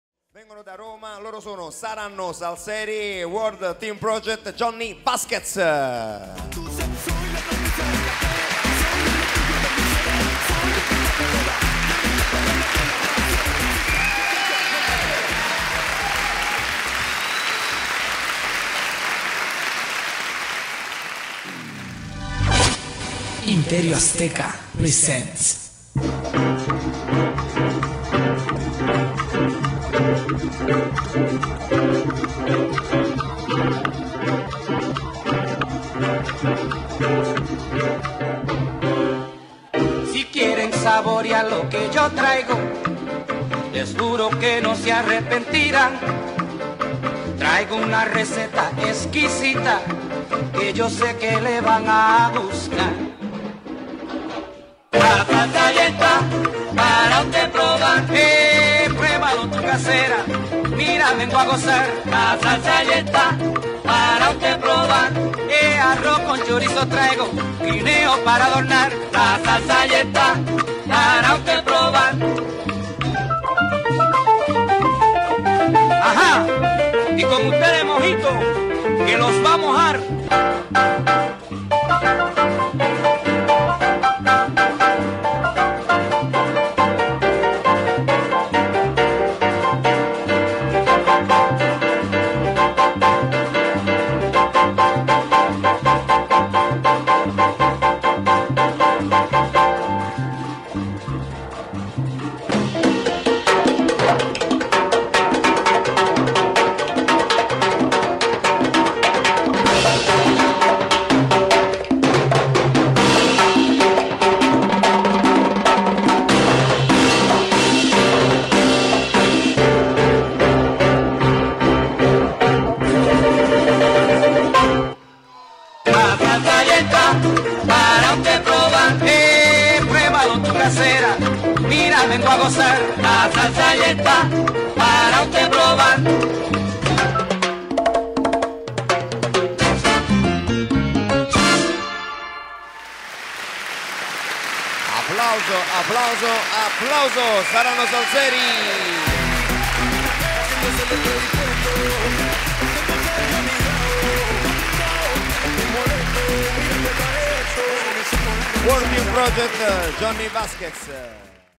SARANNO SALSERI & WTP JOHNNY VAZQUEZ
Le telecamere di movidachannel.com erano presenti alla 25ma edizione del Salsitaly Winter 2017